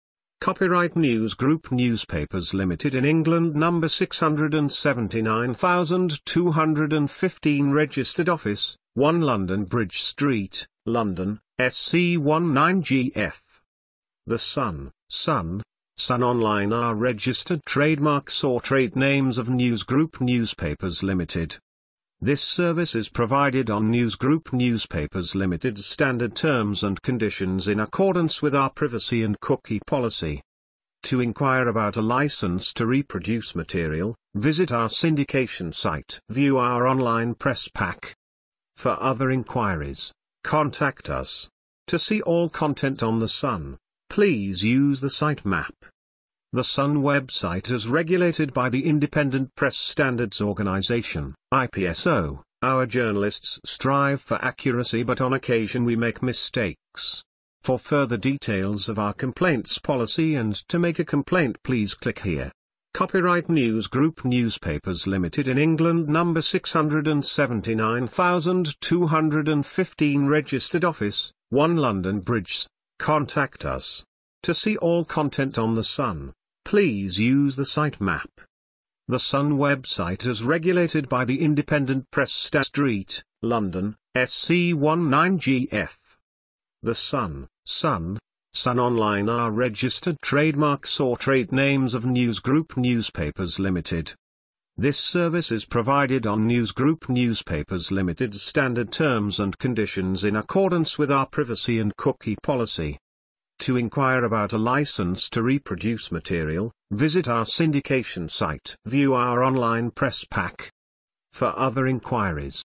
Schalke will now punish players by making them work in the club shop, according to sporting directo
Schalke will now punish players by making them work in the club shop, according to sporting director Christian Heidel Source Photo and Content: